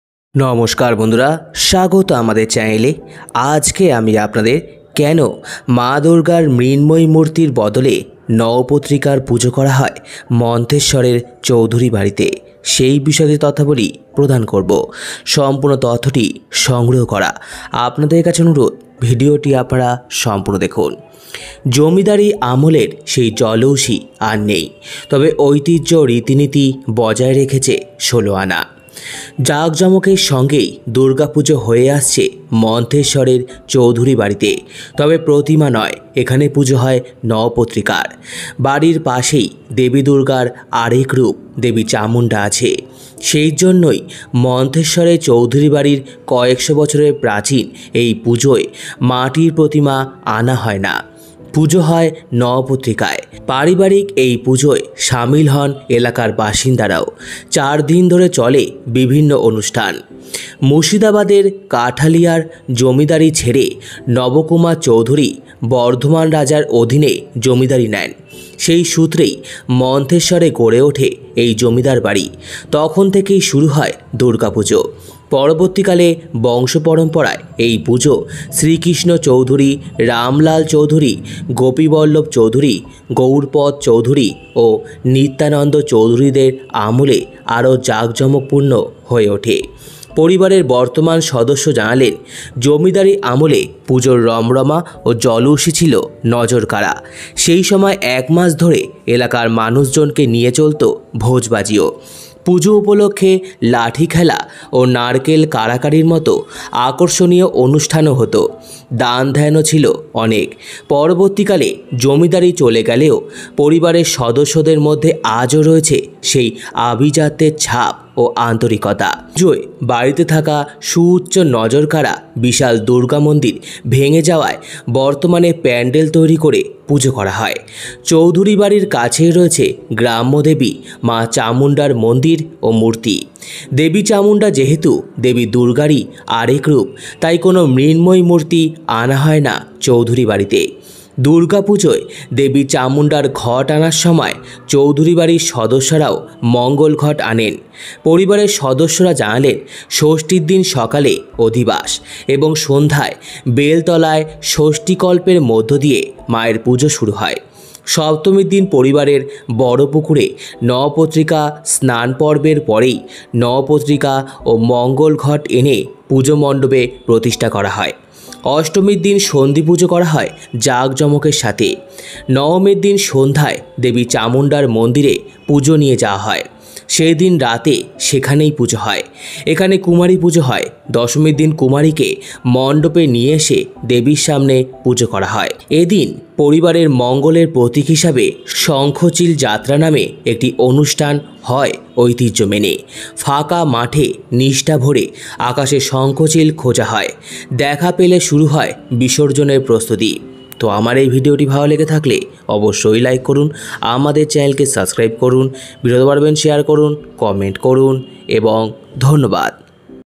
কেন মা দুর্গার মৃন্ময়ী মূর্তির বদলে নবপত্রিকার পুজো করা হয় মন্তেশ্বরের চৌধুরী বাড়িতে?
তথ্য ও চিত্র সংগৃহীত